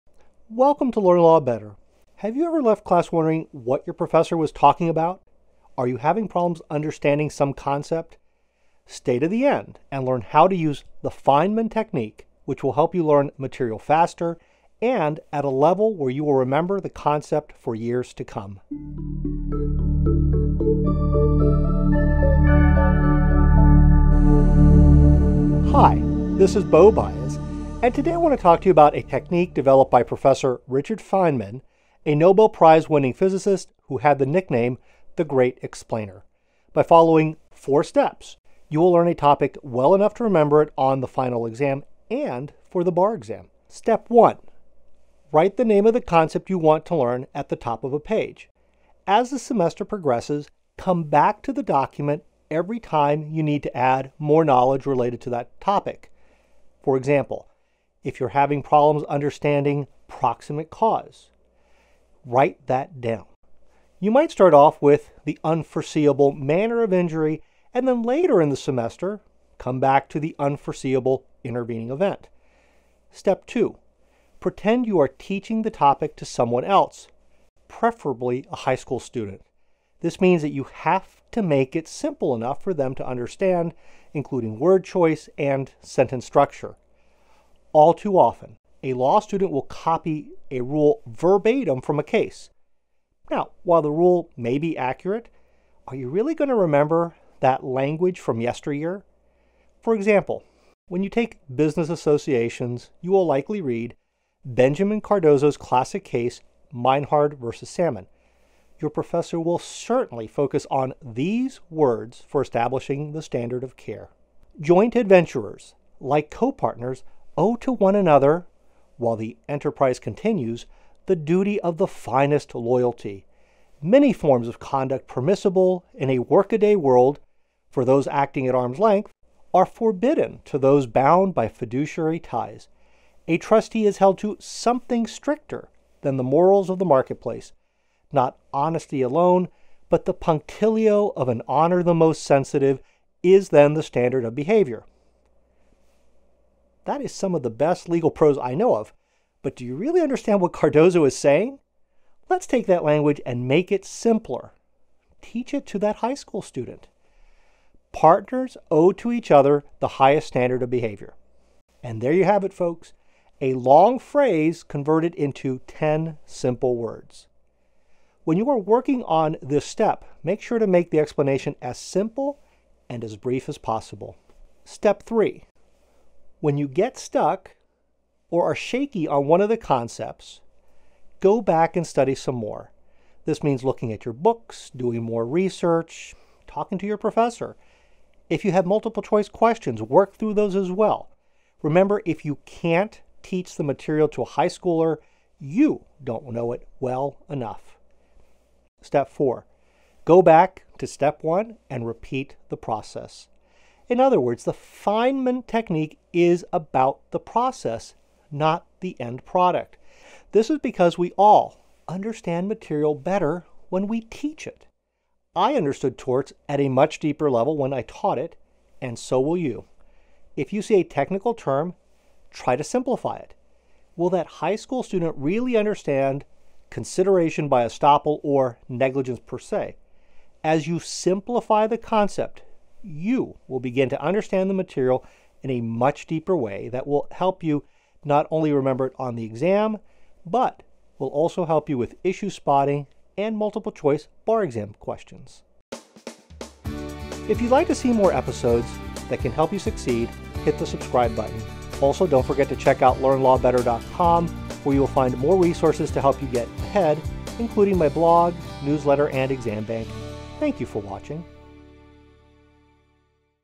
Learning Faster with the Feynman Technique
Learn law faster and better through a four step process developed by Noble prize winning professor Feynman. These steps are designed to help you think about any concept you want to learn at a deeper level.

Today I want to talk to you about a technique developed by Professor Richard Feynman, a Nobel prize winning physicist who had the nickname “the Great Explainer.” By following four steps, you will learn a topic well enough to remember it on the final exam, and for the bar exam.

Learn Law Better is designed to help law students in law school and those preparing for the bar exam.